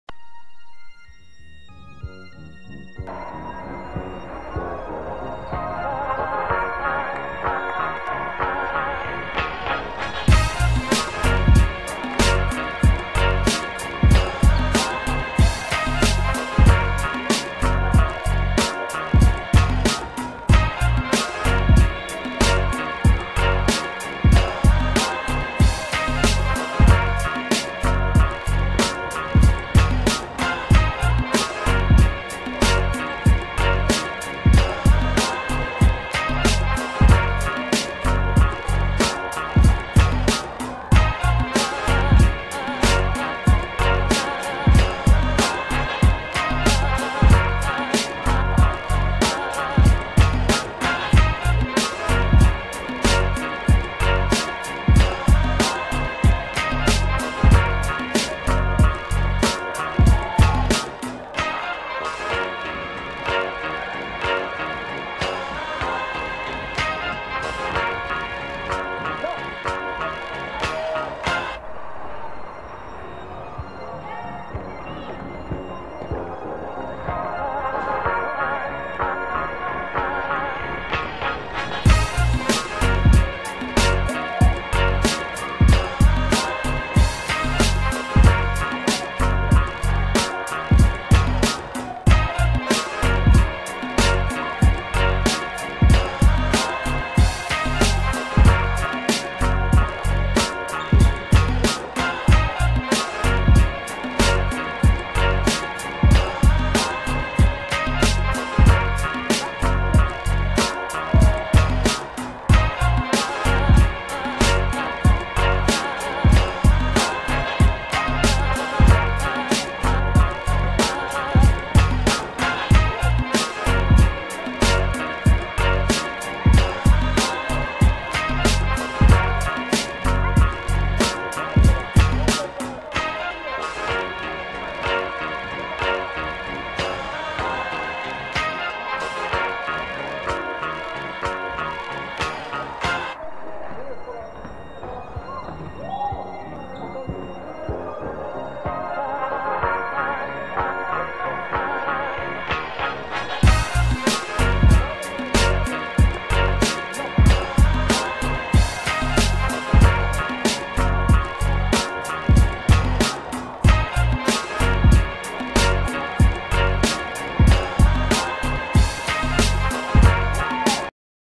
21 10 Lwy vs Kraby – Dzika Liga Junior 4
LWY 33:36 KRABY
M.Piotrowski 9pkt 13zb 6stl
M.Melon 18pkt 7zb 5stl